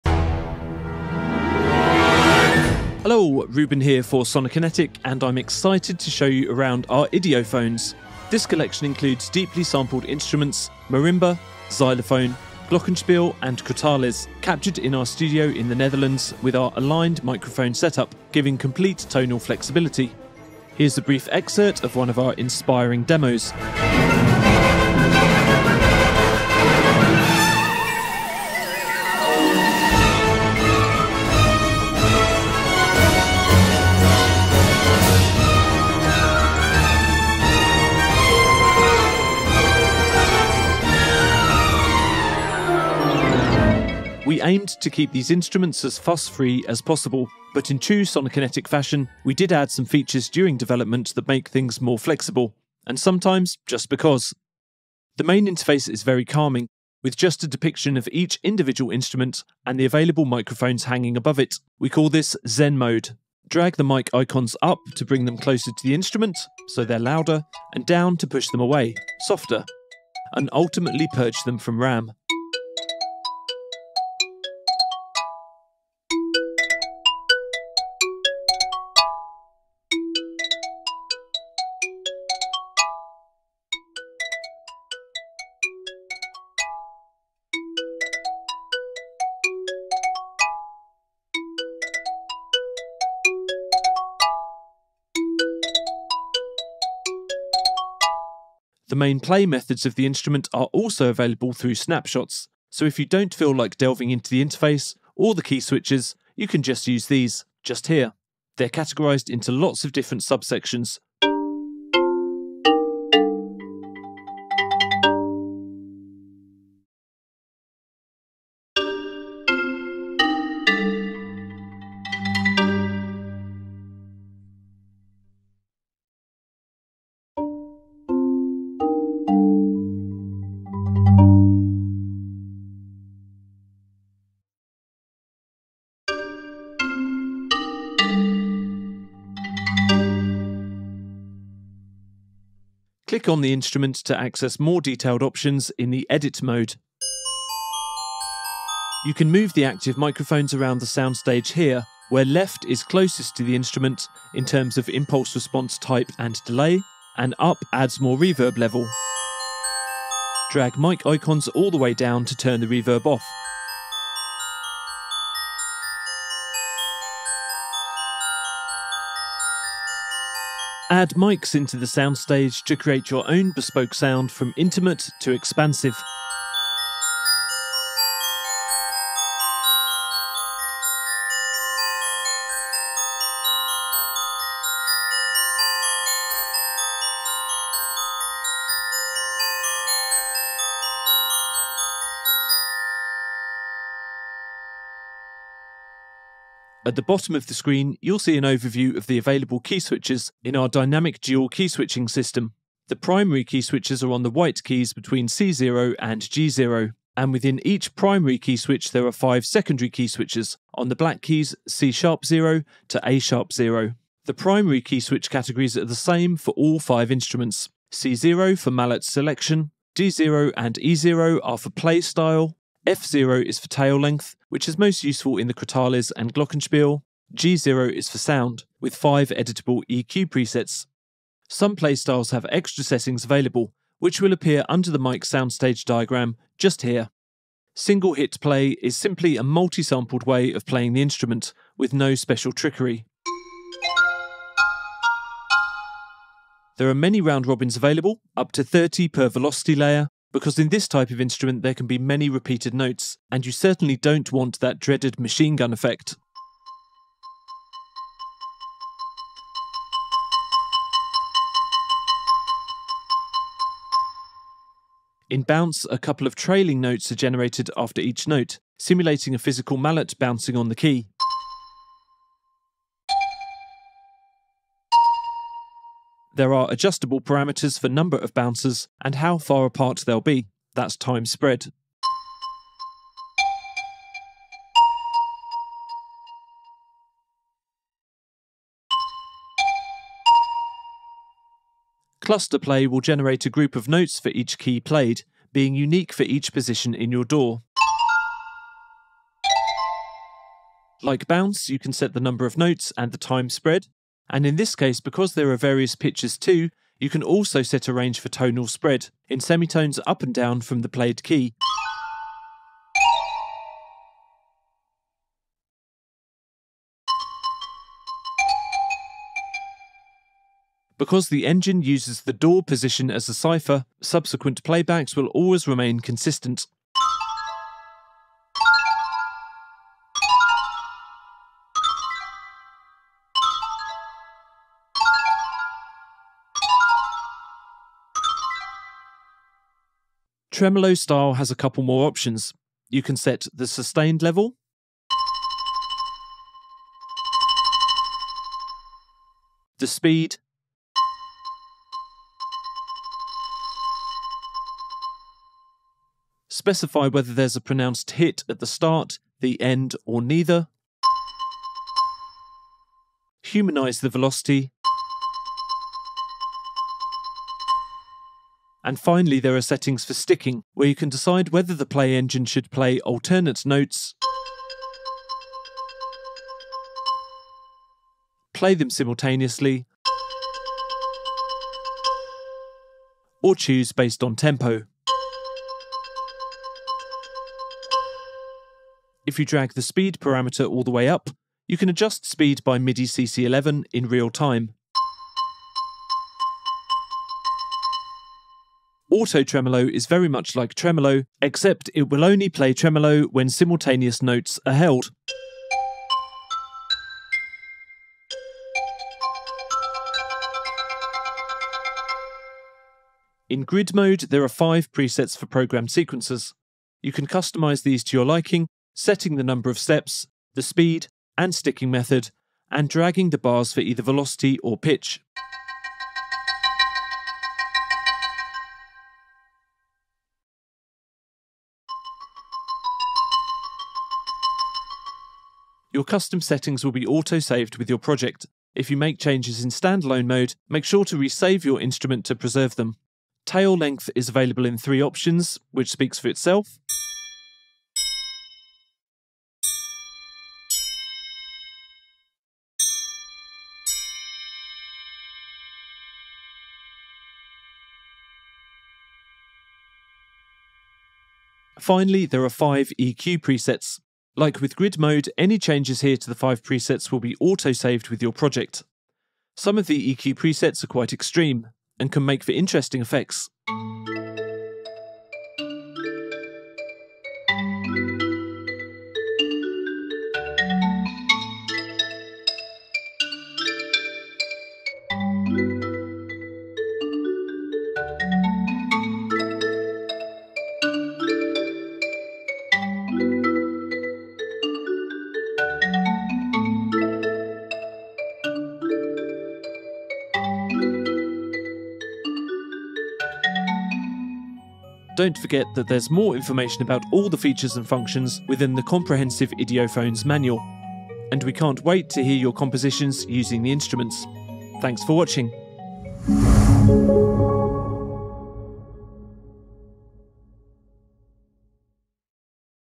Discover "Idiophones"
🎹Discover the refined sounds of our Idiophones library, featuring expertly sampled Marimba, Xylophone, Glockenspiel, and Crotales. With over 150,000 samples and 19GB of content, plus distinctive microphone positioning options, this collection offers exceptional depth and realism for your compositions. Compatible with 🎵KOMPLETE KONTROL & KONTAKT PLAYER, it's an essential tool for serious musicians and composers